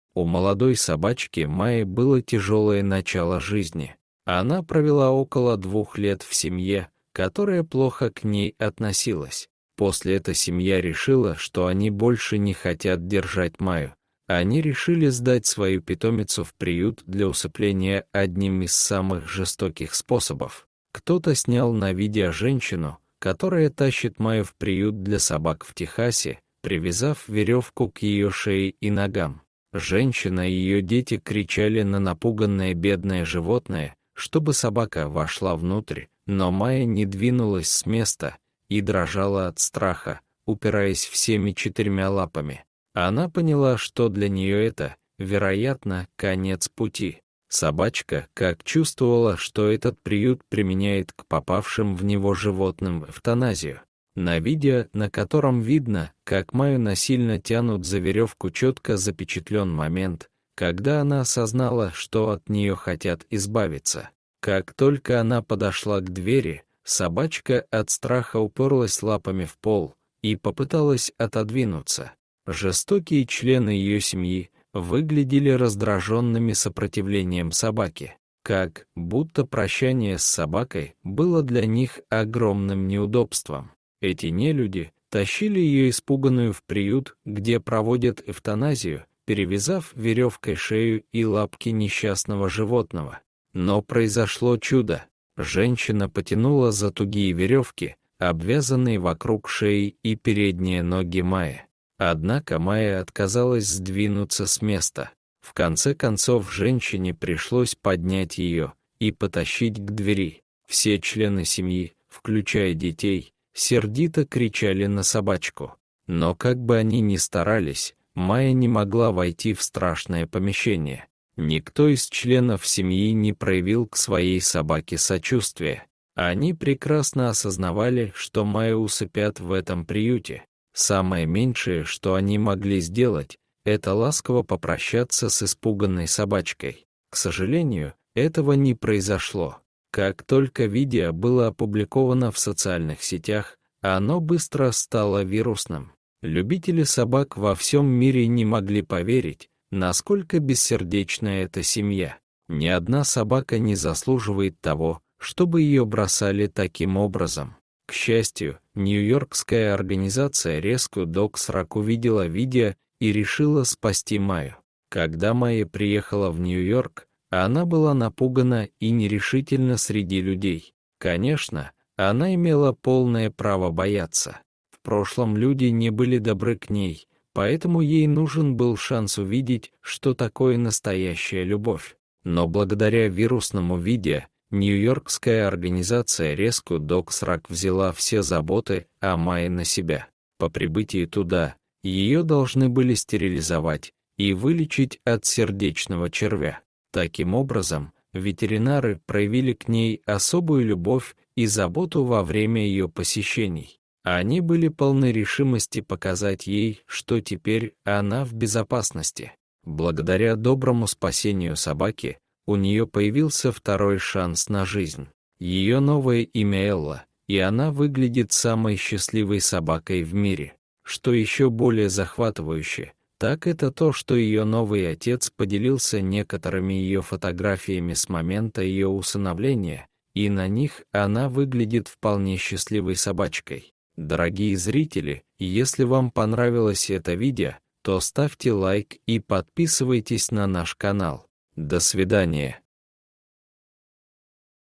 Собачку вели на усыпление, но в последний момент произошло невероятное!
У молодой собачки Майи было тяжелое начало жизни. Она провела около двух лет в семье, которая плохо к ней относилась. После эта семья решила, что они больше не хотят держать Майю, они решили сдать свою питомицу в приют для усыпления одним из самых жестоких способов.